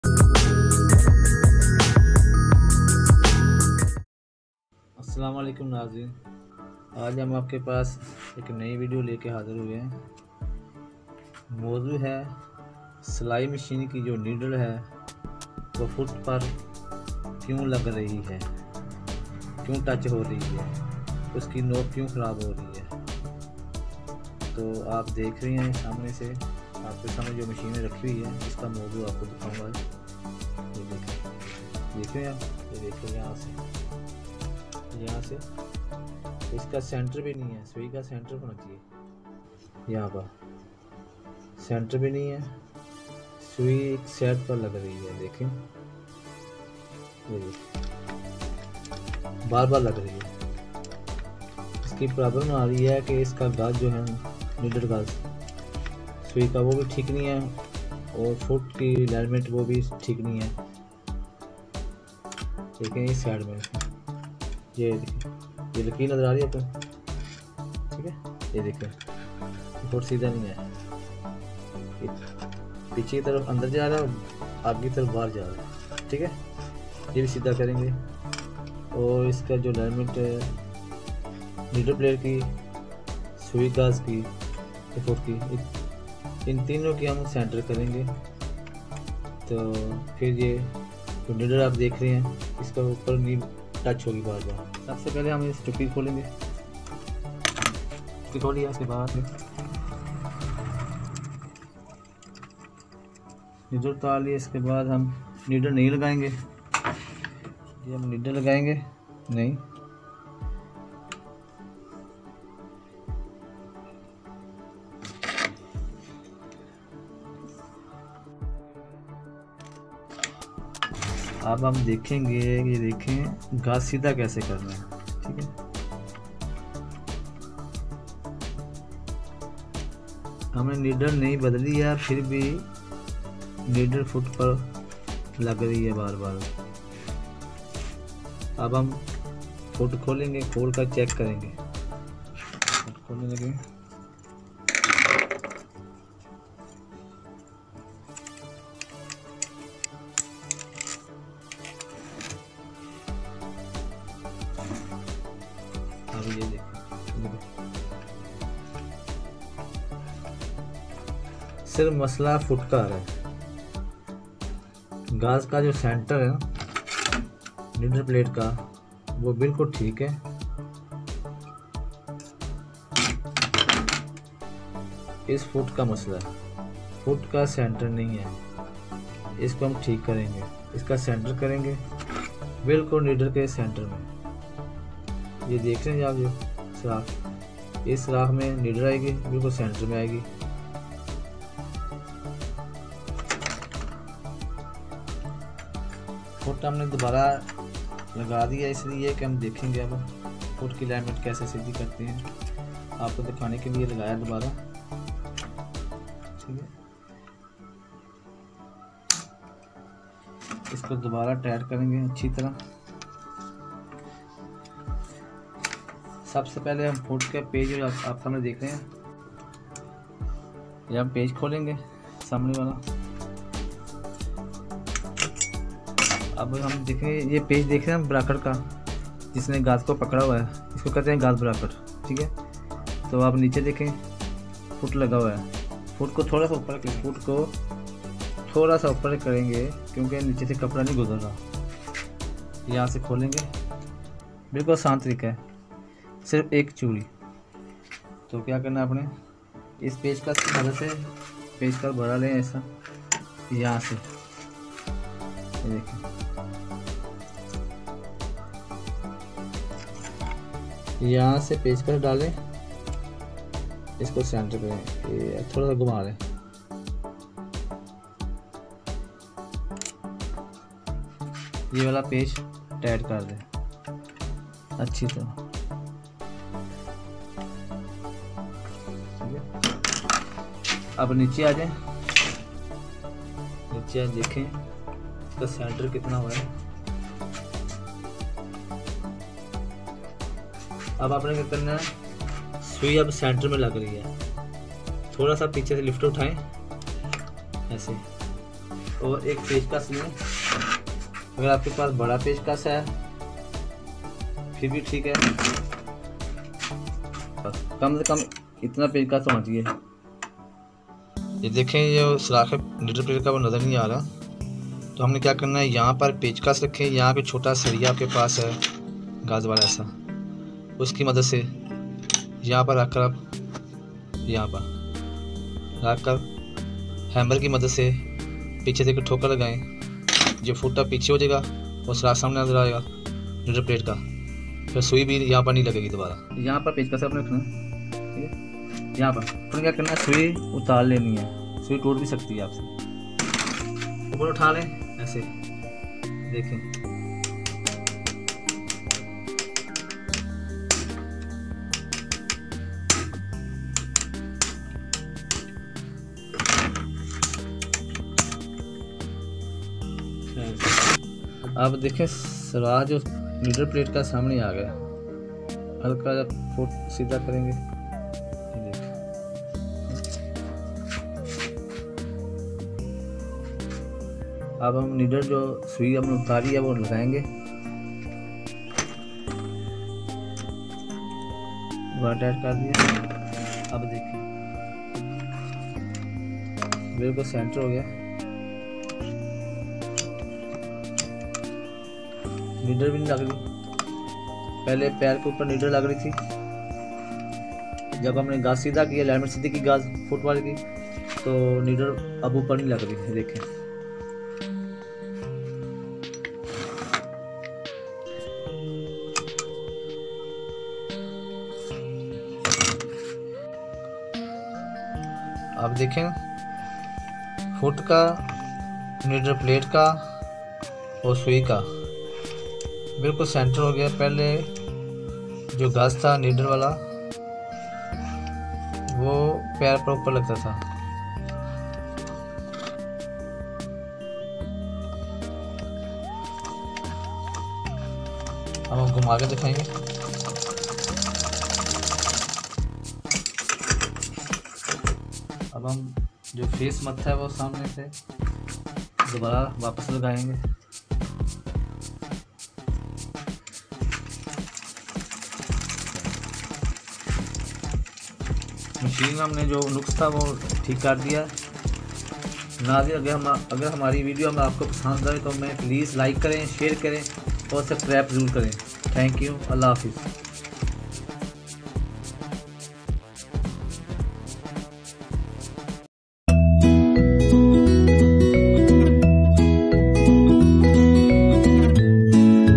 NEEDLE HITTING PRESSER FOOT | LEARN HOW TO SOLVE THE PROBLEM AT HOME | DIY TUTORIAL
How to Re-Align the Needle and Presser Foot on Sewing Machine